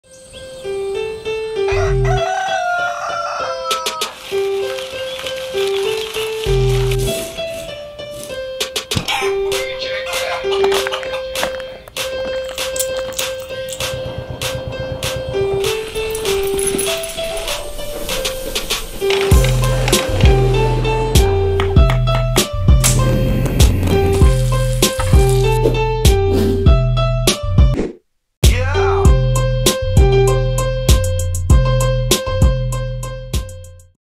whole foods ad - sound redesign
A Whole Foods ad where I redone all the audio for the ad. I sourced the music and sound effects then I put together a new edit for the audio.